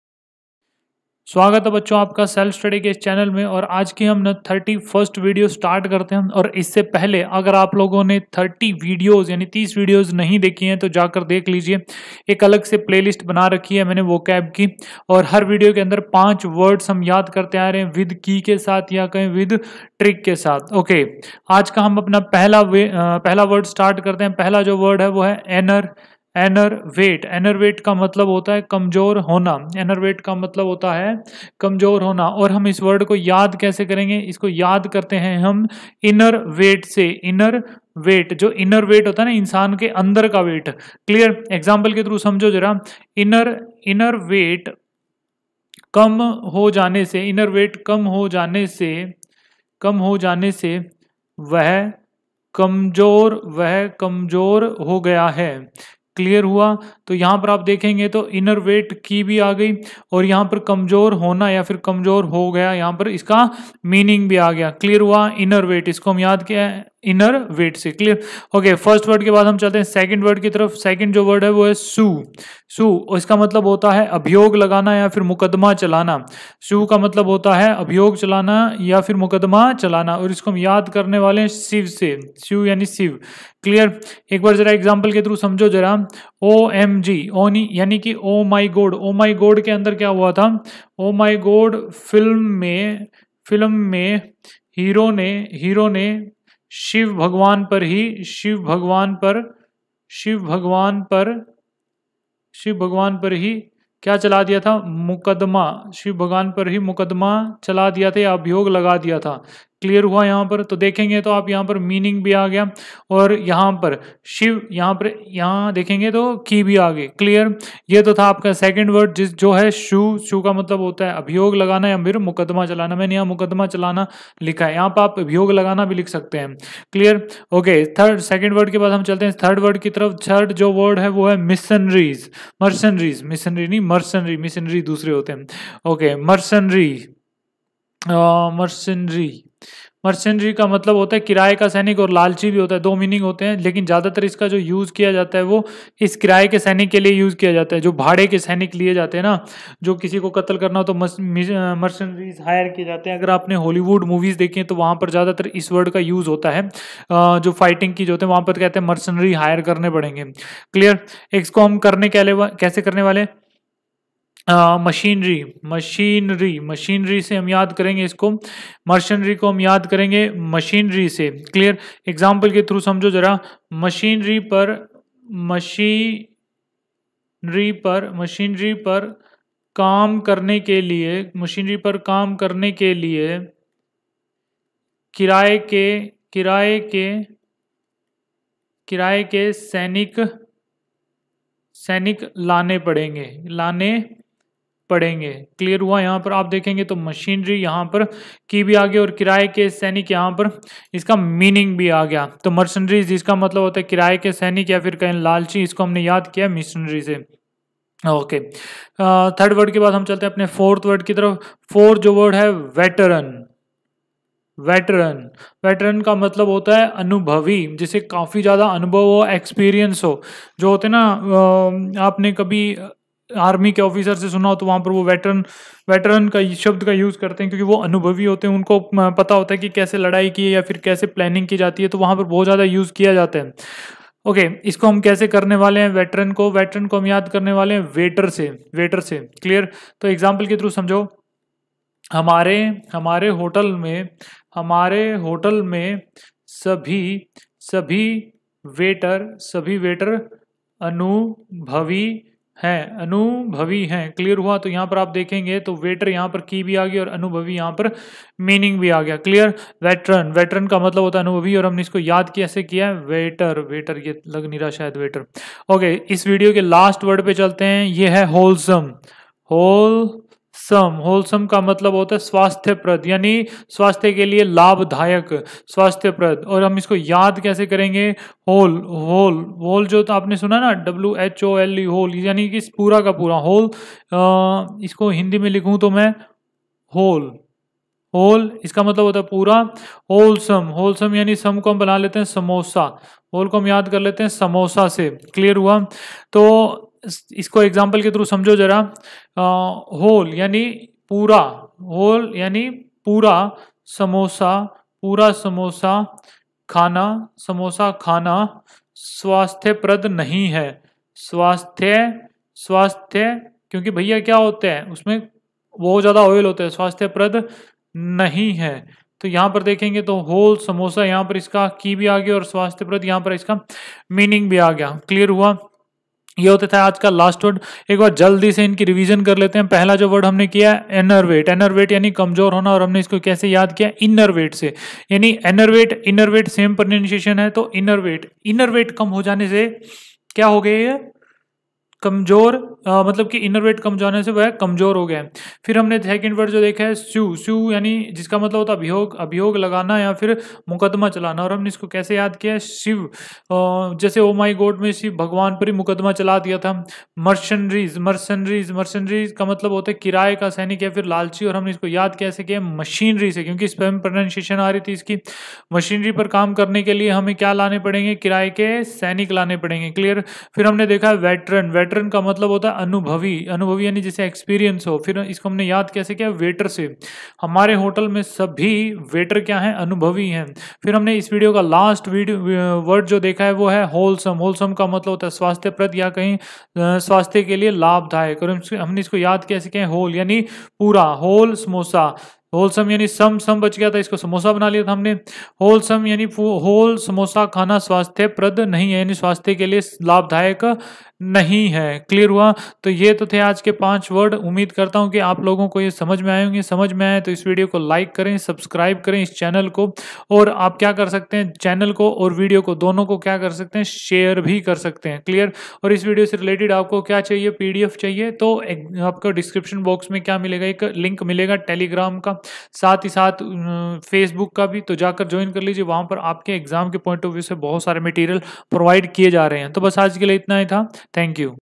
Daily 5 Words Learn With Trick |Vocabulary Video 31 | हर रोज 5 शब्द अंग्रेजी भाषा के सीखेSelf Study
Learn 5 English words in easy way (By Trick). Build your Vocabulary for any competitive exam (CGL, CHSL, CDS, ARMY, BANK)